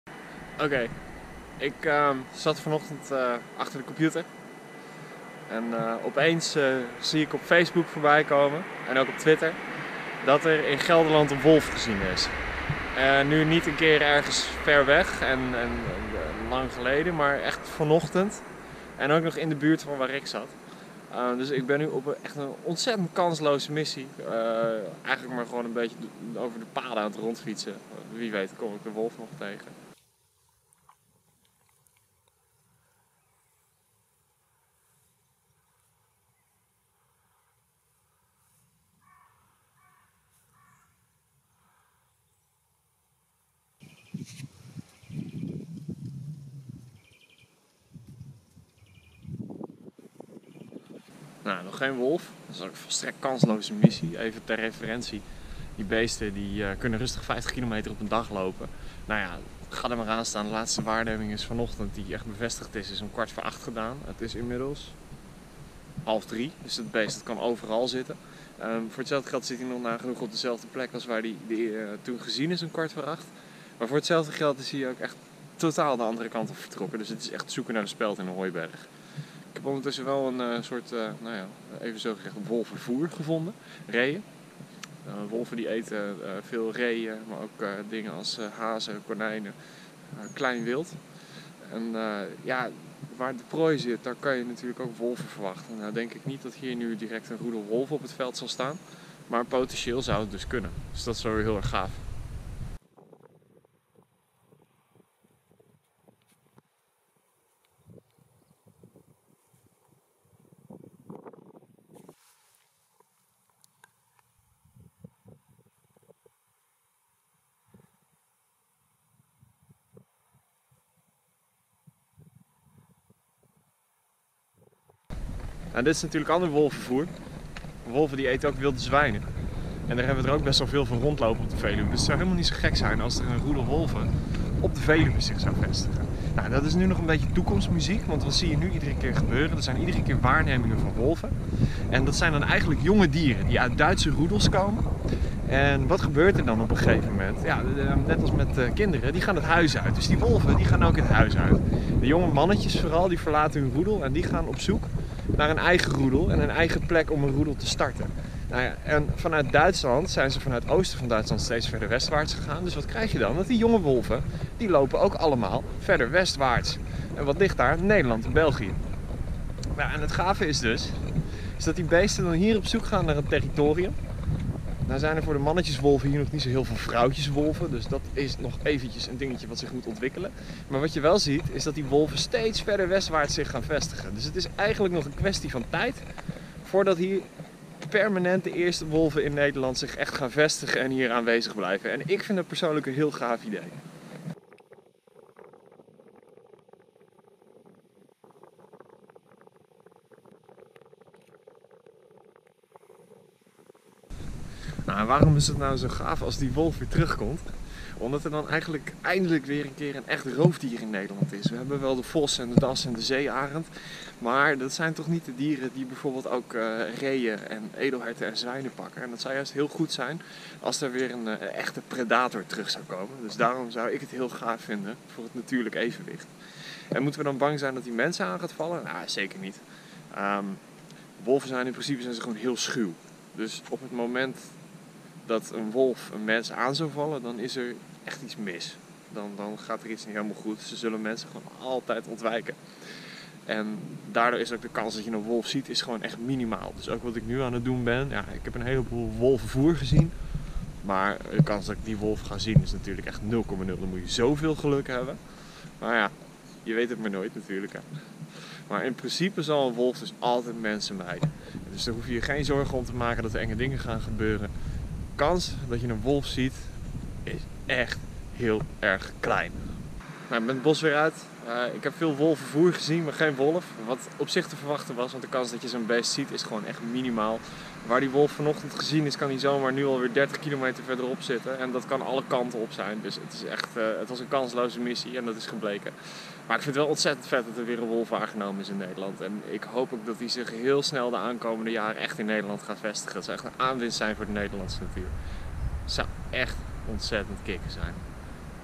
Vlog #32: Er is een wolf gezien in Gelderland!
Er is een wolf gezien in Gelderland, vlakbij mijn favoriete wildzoekgebieden! De wolf zoeken is kansloos kan ik uit ervaring zeggen maar wat is het gaaf dat er weer wolven naar Nederland komen! Welkom wolf!